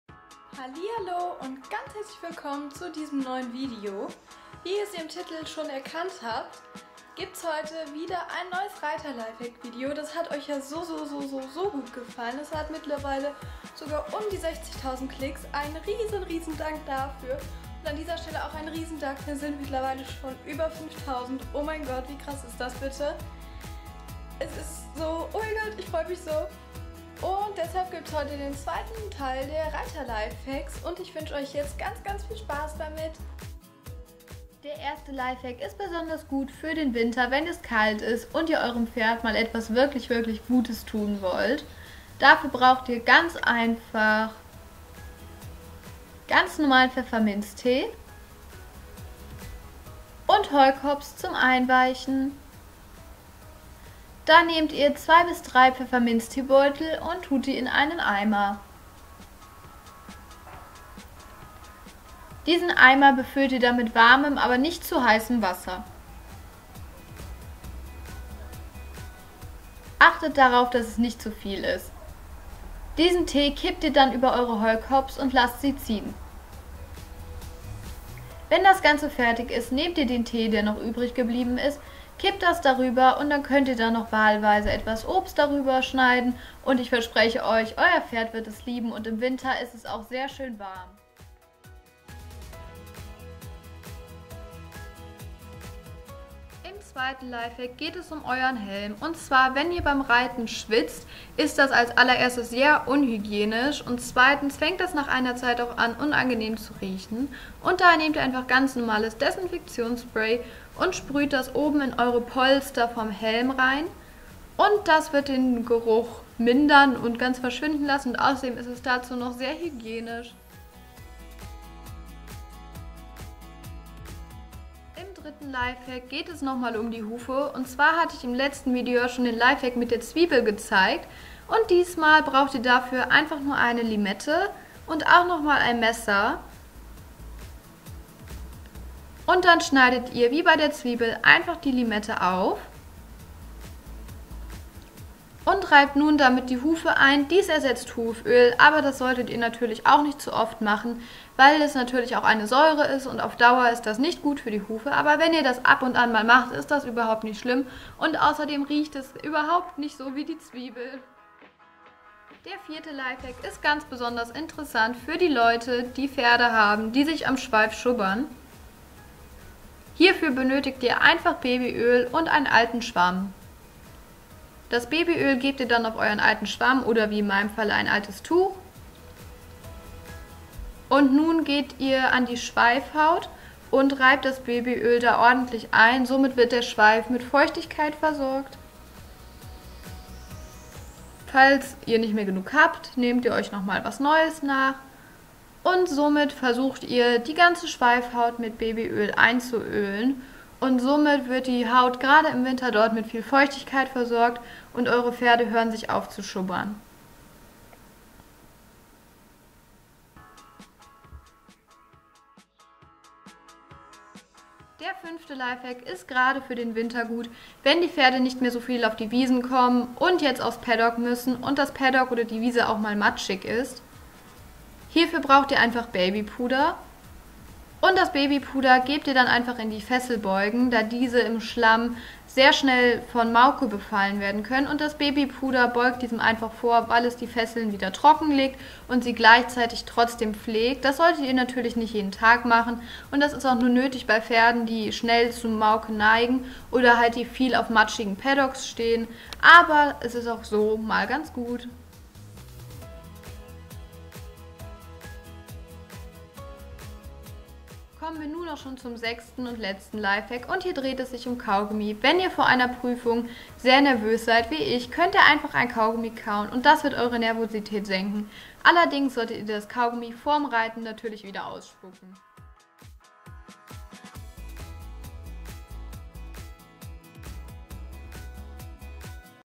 5+1 REITERLIFEHACKS | Reiterlifehacks für den Winter | Lucky Merle
Heeyy
heute gibt es endlich den 2.  Teil der Reiterlifehacks! Ich hoffe das euch das Video genauso gut gefallen hat wie das letzte !